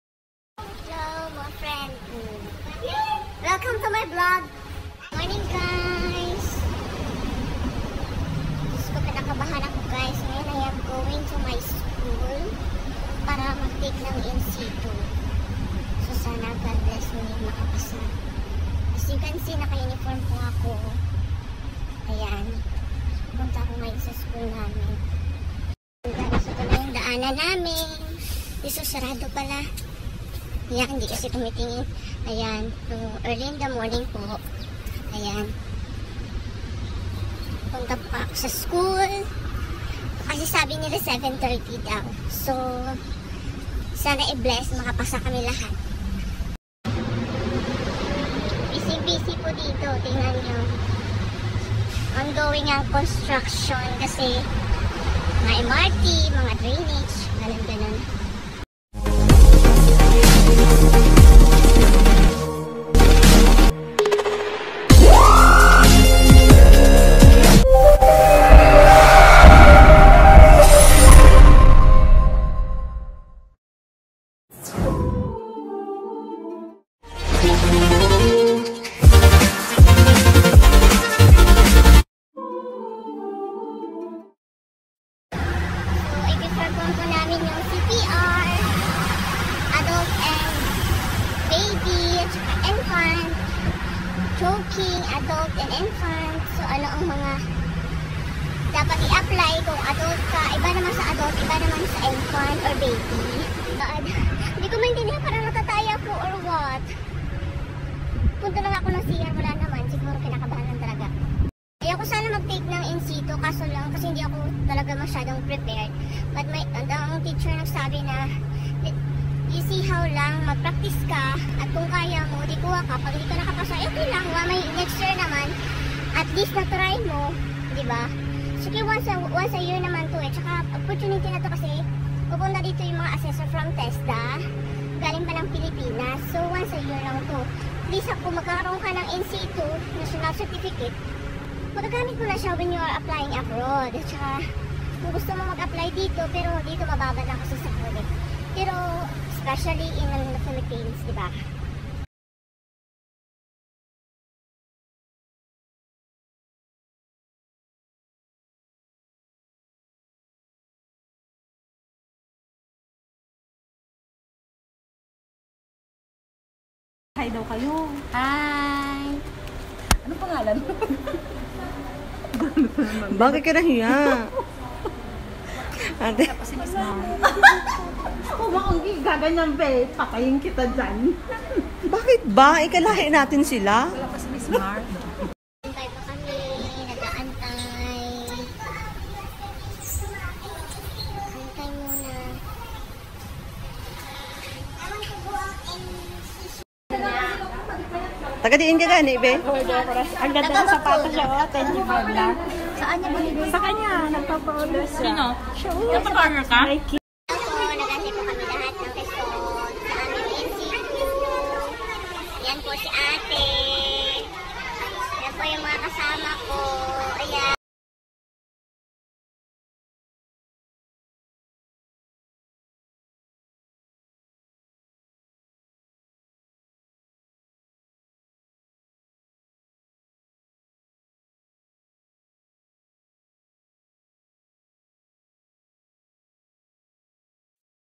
assestment is real
TESDA NC2 Singapore
Caregiving course
proper hand washing
CPR ER ADULT AND INFANT
# Rescue CHOKING FOR INFANT
Baby bathing ( proper procedure)
Bed bathing
Bed making occupies&unoccupied
Bed Mobility
preparing meals for toddlers and diabetic patient ( hyperactive child )